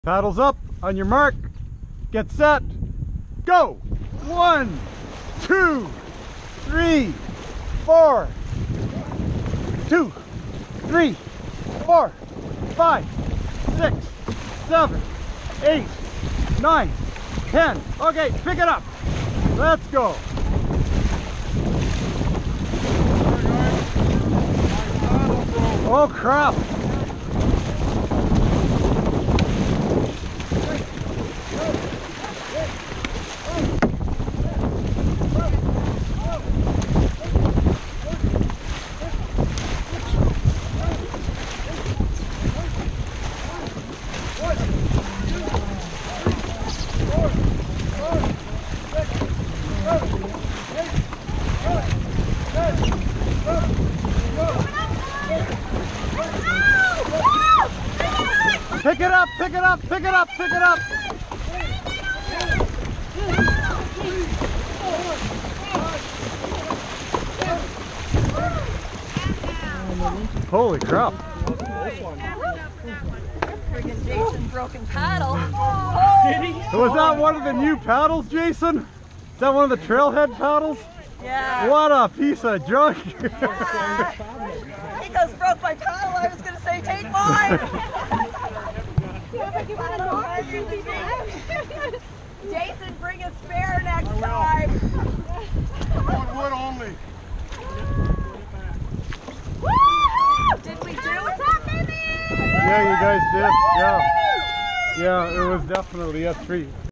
Kilmarnock Enterprise wins Rideau Paddlefest Smiths Falls 2016short
One day festival in Smiths Falls, ON celebrating the flat water of the Rideau Canal waterway, a UNESCO World Heritage Site!